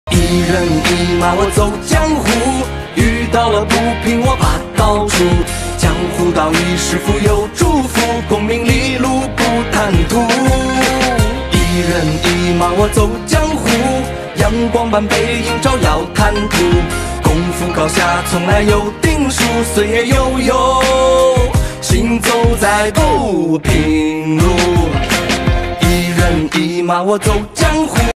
A woodworking machine specially used for cutting
CNC cutting machine is a kind of machine specially used for cutting plates, which is suitable for batch production of enterprises, with simple operation and stable operation